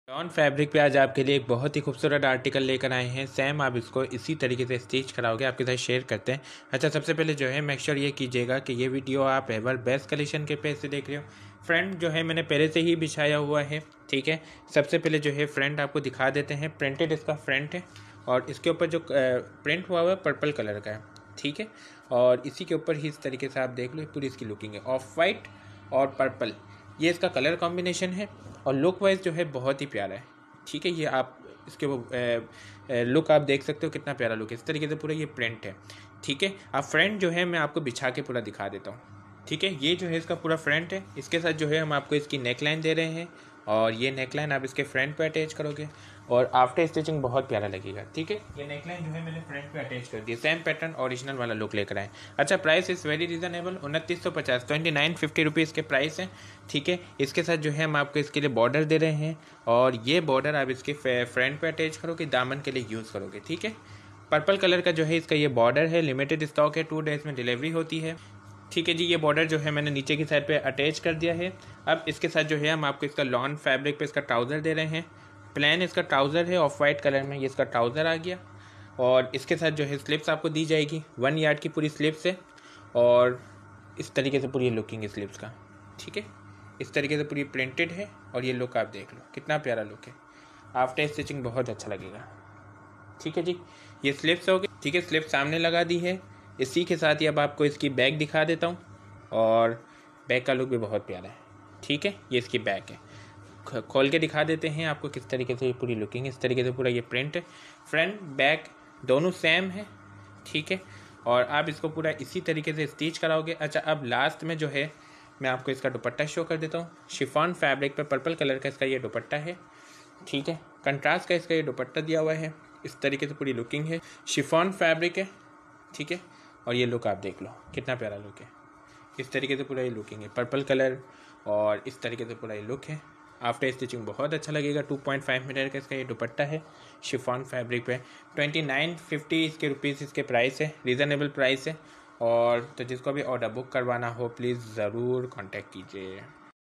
Luxury Lawn😍🔥🔥Black ColorNew Arrival 🥰💥💥Beautiful Colour 💐Price:2850/- | Ever Best Collection
Luxury Lawn😍🔥🔥Black Color
New Arrival 🥰💥💥
Beautiful Colour 💐 Mashaallah
Price:2850/- Fixed With 250/- Delivery Charges Apply Fixed rate
Fabric Lawn
Dupatta Chiffon
Lawn Trouser
Front lenght 45+
Front width 39+
2.5 meter Dupatta
2.5 meter Trouser